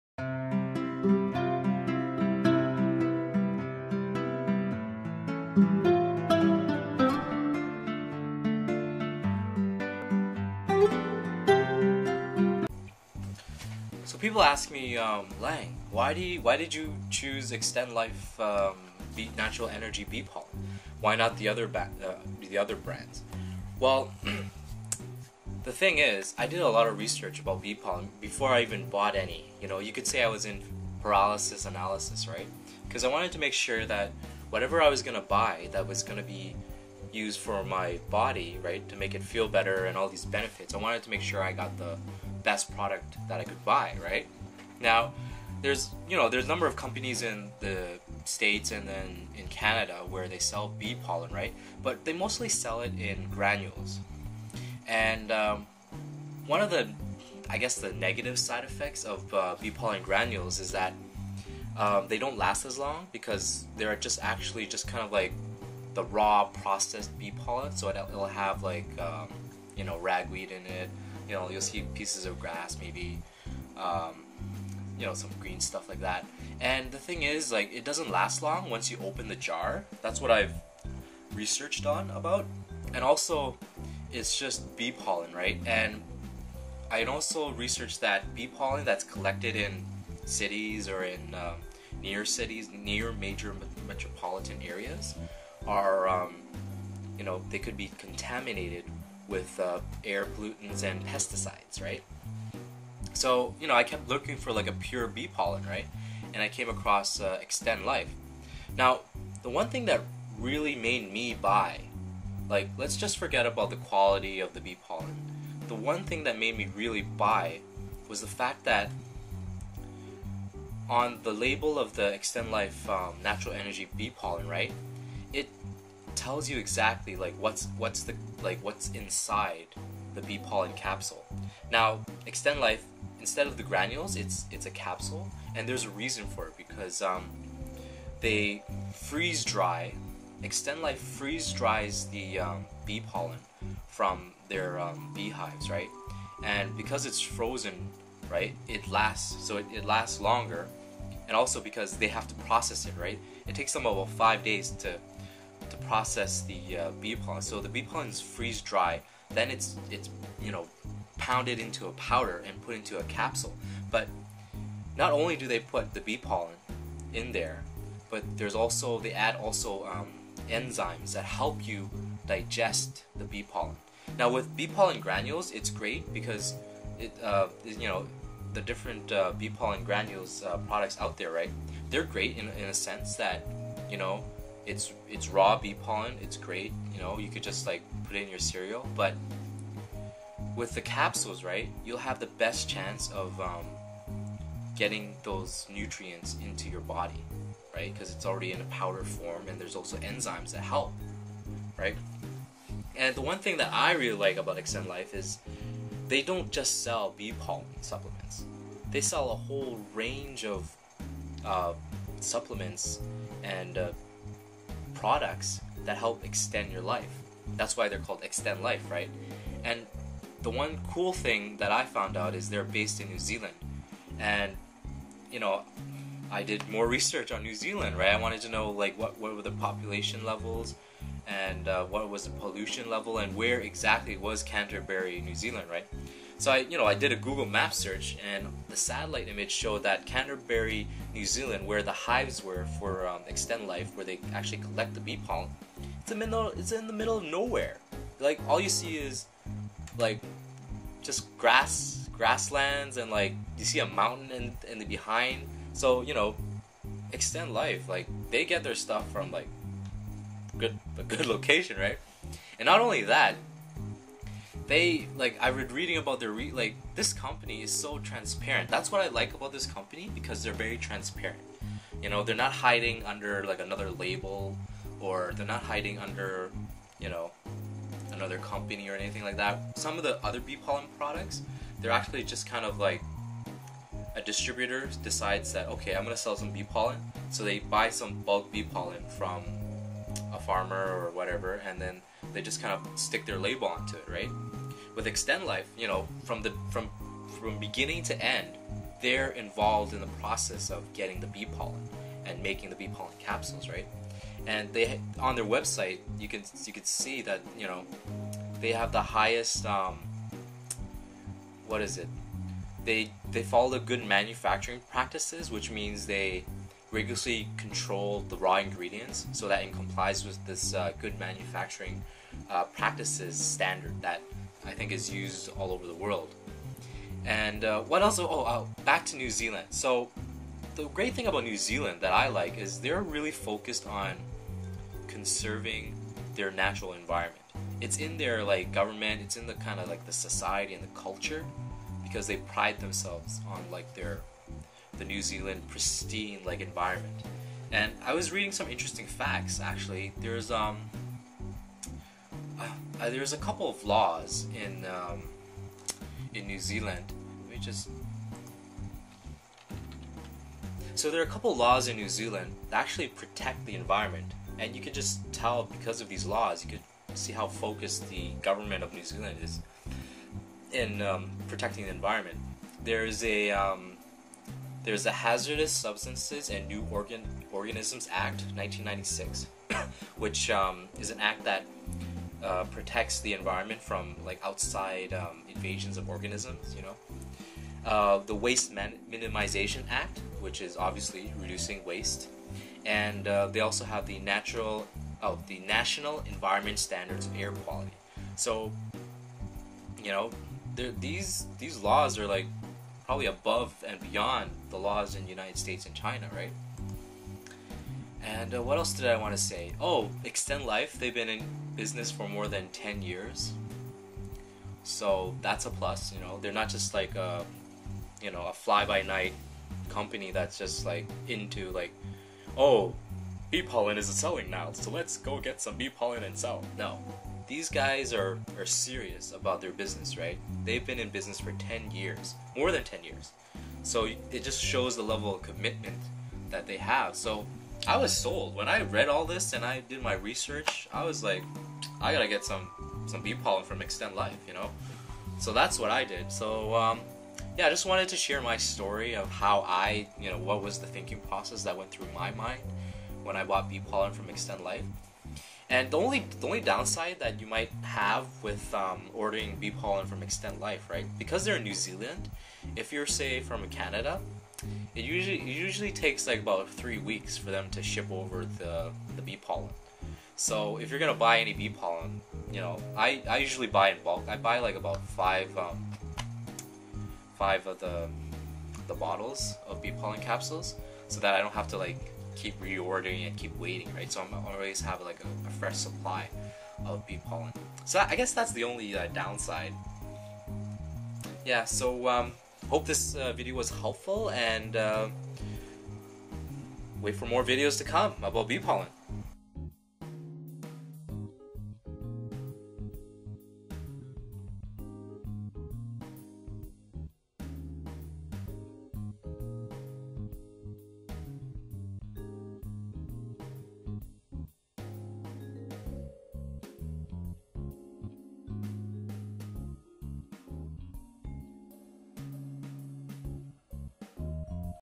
Why I chose Xtend-Life Bee Pollen ? Learn the mistakes I made so you don't!
Bee Pollen Buy, Learn Why I chose bee pollen from Xtend-Life!

Bee Pollen has numerous benefits.  I love taking bee pollen in capsule form because its simple and easy, I don't have to worry about cooking or preparing the bee pollen overnight as that is the case with bee pollen granules.